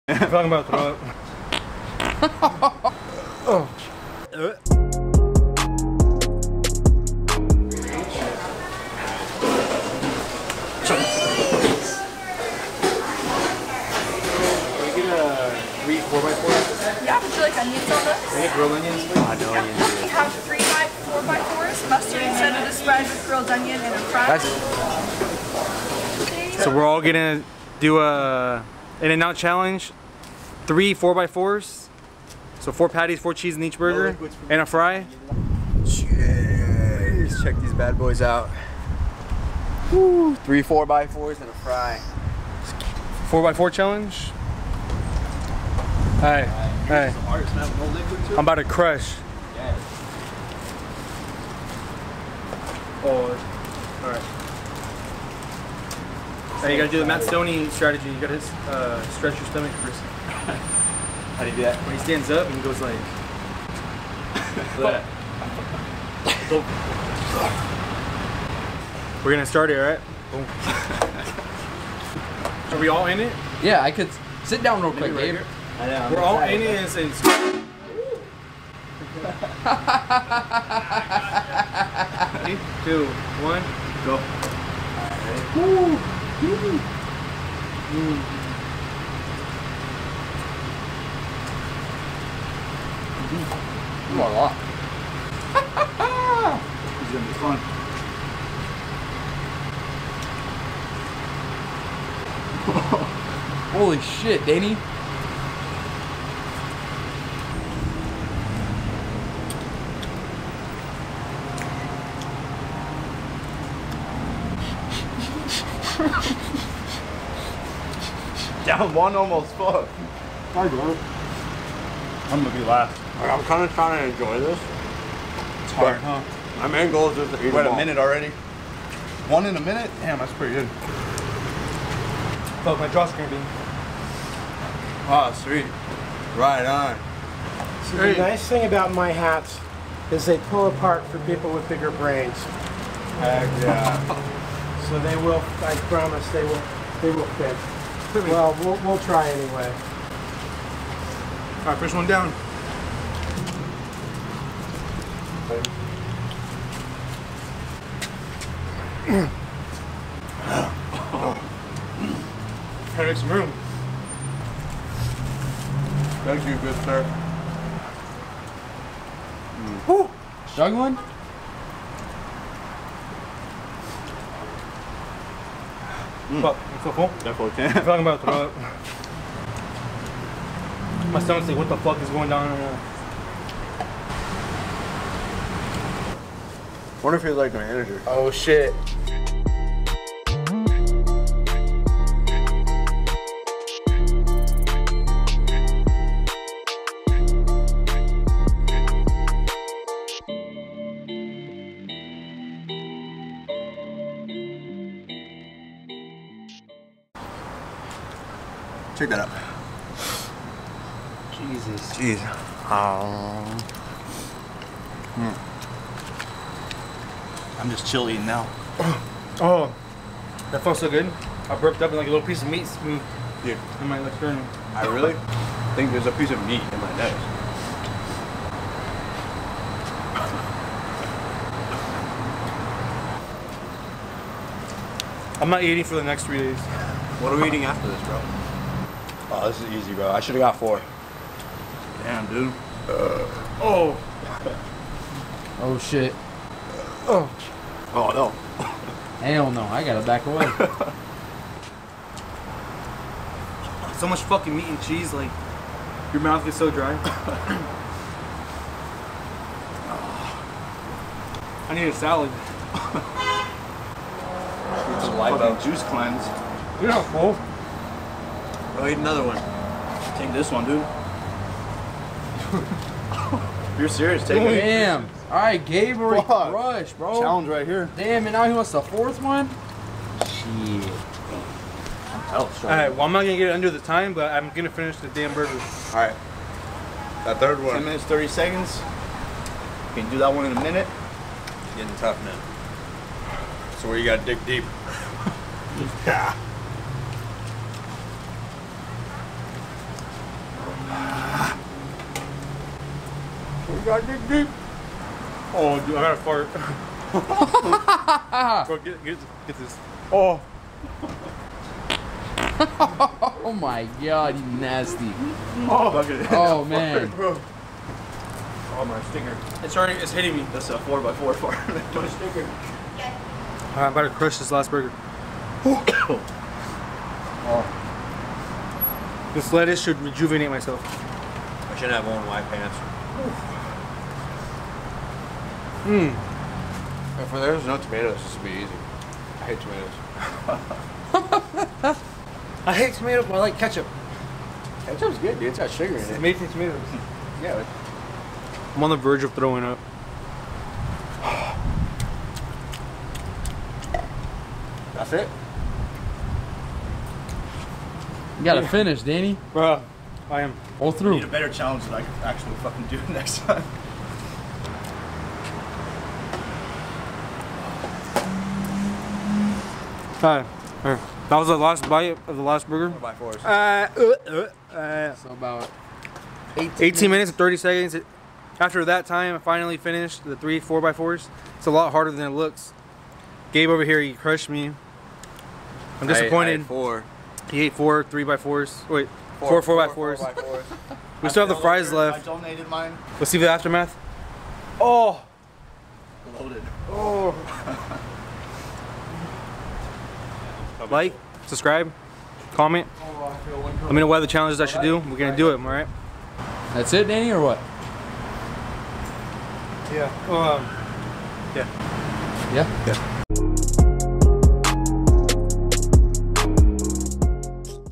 IN-N-OUT 3x4x4 Challenge
We challenged ourselves to eat 3 IN-N-OUT 4x4 Burgers in 10 minutes! Never again.